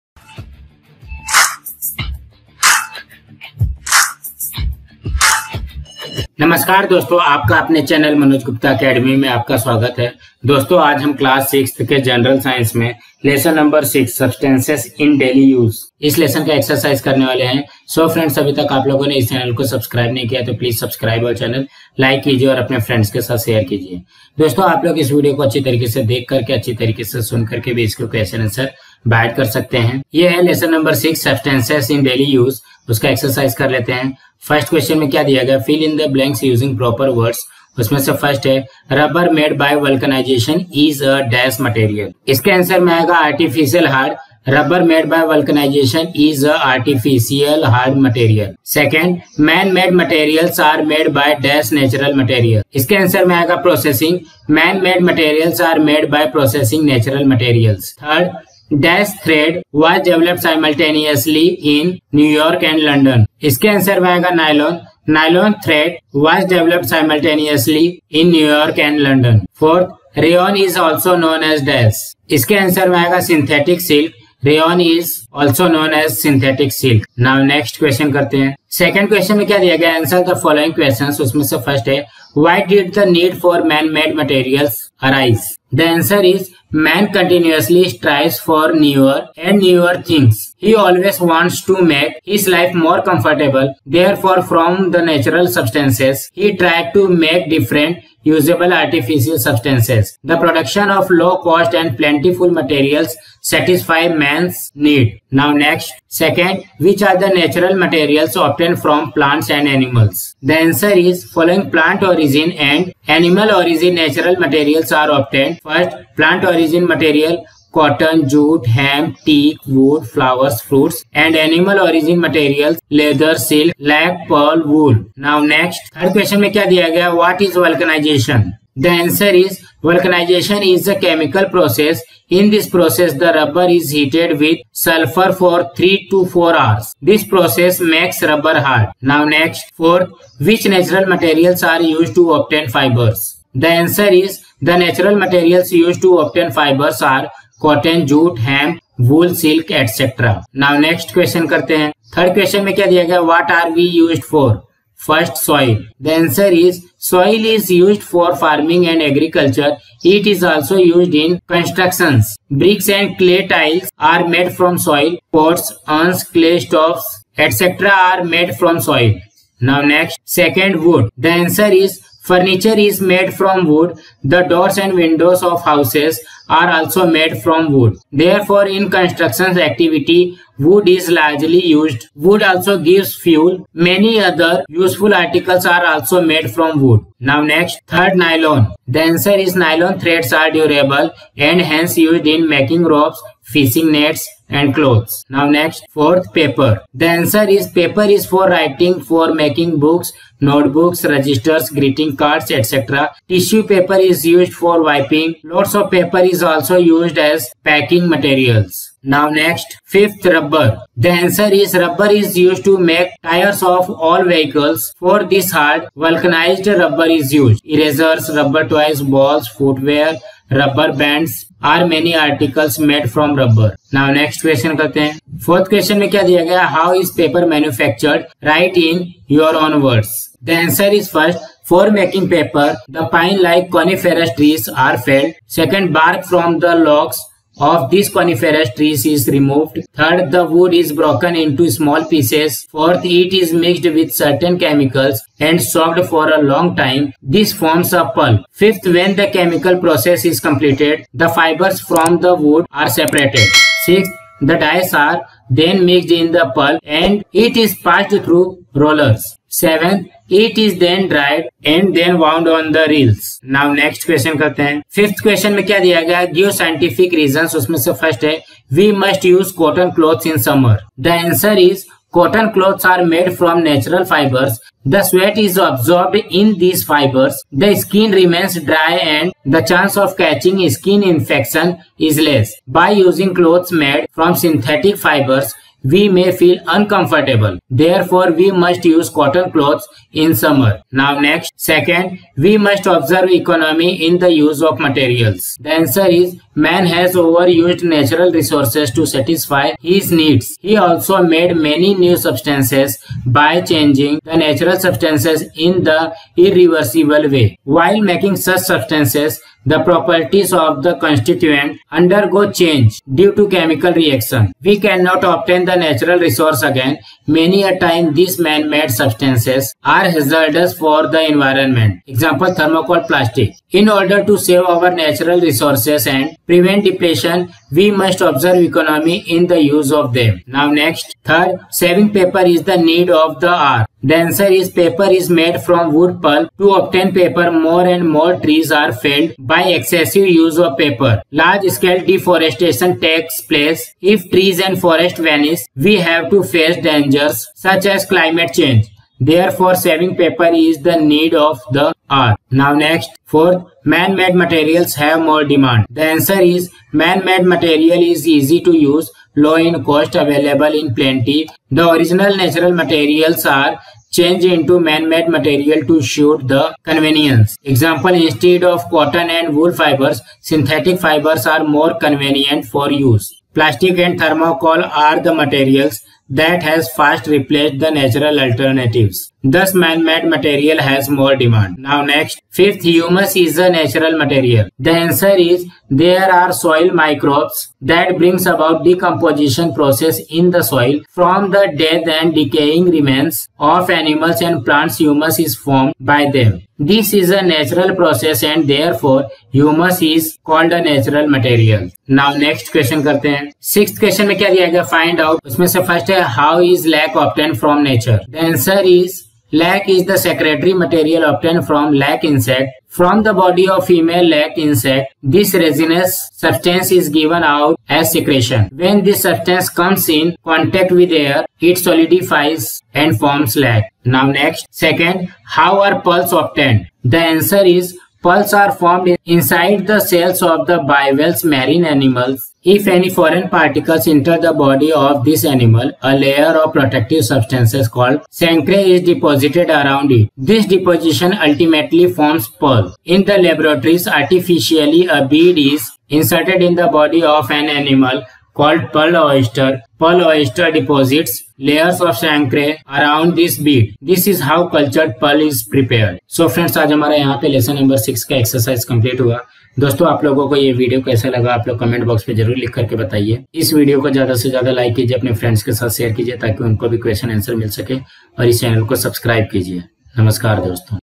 Exercise Lesson No 06-Substances in daily Use Class 6th General Science English Medium State Board
Hello Friends

Q.A.(Exercise) Lesson NO 06-Substances in daily Use Class 6th General Science English Medium  Maharashtra Board

class 6th General Science Lesson No 06-Substances in daily Use